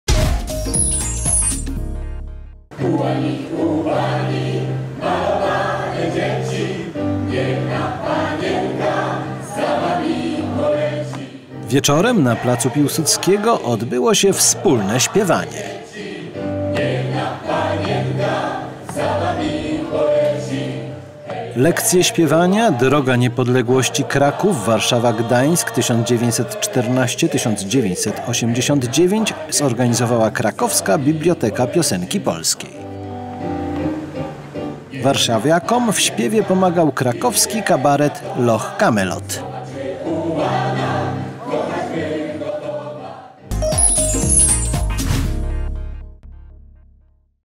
Pieśni Niepodległościowe
11 listopadowy wieczór warszawiacy spędzili, śpiewając pieśni niepodległościowe na pl. Piłsudskiego.